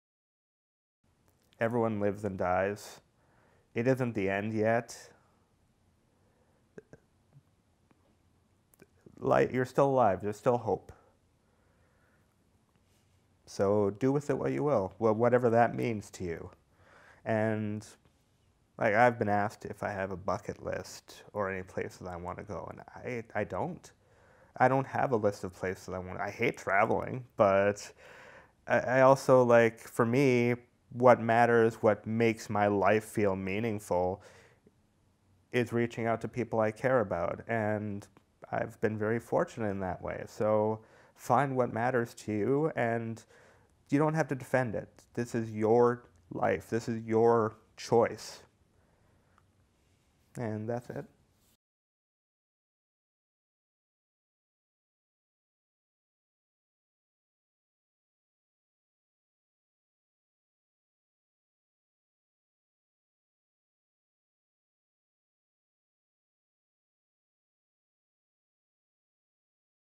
Find what matters to you
In this video by Canadian Virtual Hospice, Dylan talks about living with advanced illness and the need to find out what matters to you and makes life meaningful.